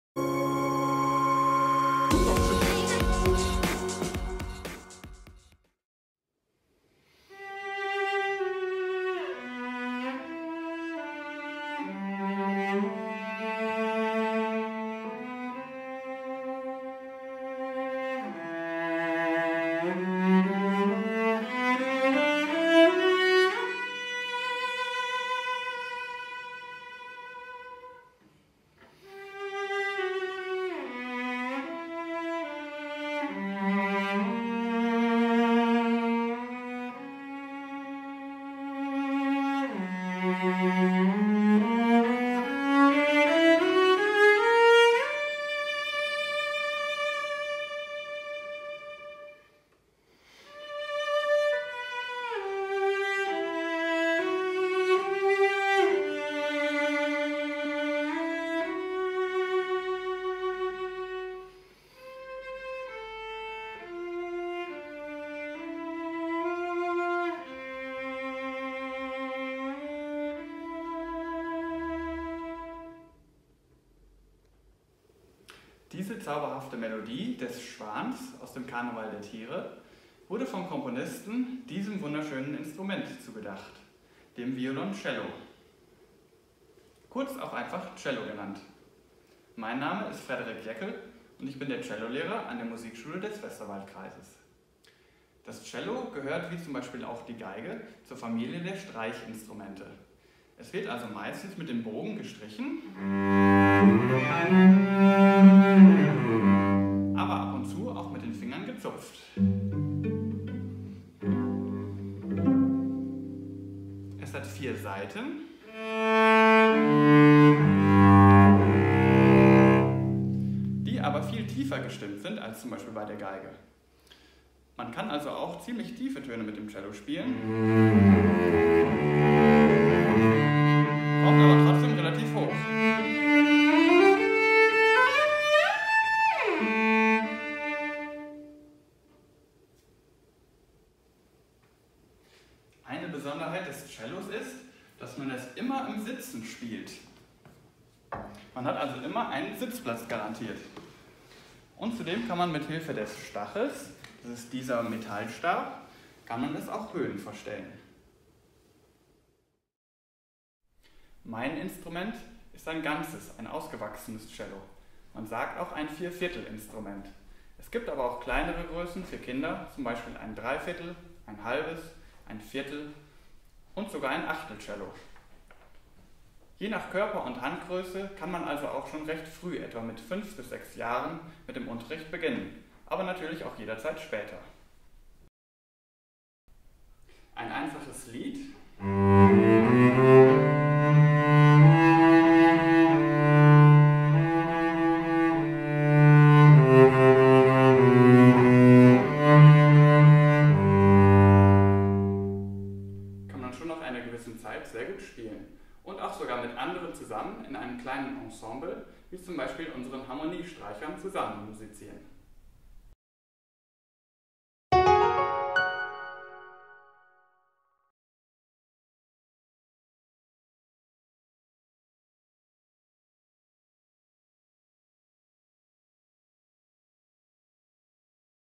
KMS Westerwald Video Violoncello 2022
Das Violoncello wird von unserer Lehrkraft Frederik Jäckel präsentiert.